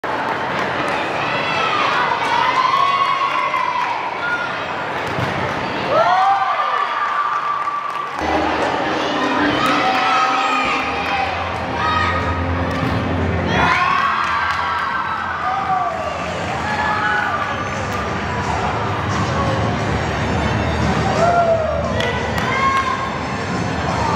Giselle Leyvas 1st Place Vault 9.8! California Classic 2023 Wildfire Gymnast Level 7
1st Place Vault 9.800. 3rd AA 37.275.